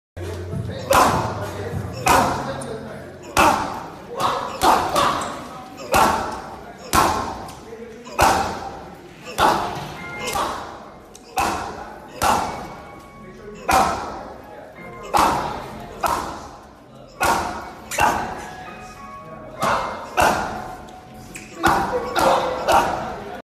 Jose "Rayo" Valenzuela prepares for his bout September 28th at the Staples Center
Video by Rainbow